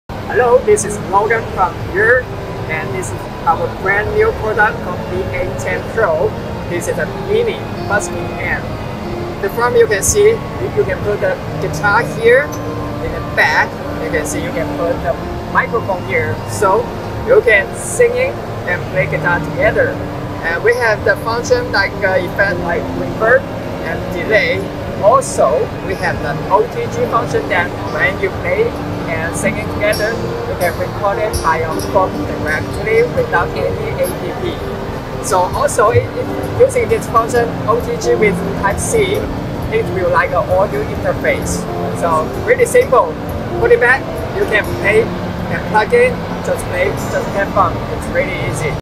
YUER BA-10Pro Mini Busking Amp Quick Review in Shanghai Music Show
Thanks for the video by Federico.
This is a special and new product called BA-10Pro Mini Busking Amp.
You can sing and play guitar in a palm size amplifier now!
 -Palm Size
-singing and playing guitar together
-Vocal cut function can cut the vocal of the song when you are using bluetooth to play song
-Guitar effect: Delay and Reverb
-Vocal effect:Reverb
-Battery:4000mAh
-It can use like a audio interface when you are doing instagram live ,facebook live,youtube live or any other live streaming platform
-Guitar for 6.35mm plug and Mic for 6.35mm plug